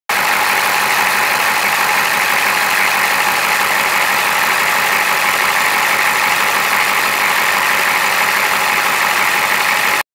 1987 Ford F350 flatbed pickup truck for sale at auction | bidding closes December 4, 2018
1987 Ford F350 flatbed pickup truck
50,200 miles on odometer
6.9L V8 OHV 16V diesel engine
Tilt
AM/FM
Manual windows and locks
Vinyl interior
Running boards
8'10'L x 92"W flatbed
8'L booms
No winch
Ball hitch
235/85R16 tires
Kansas title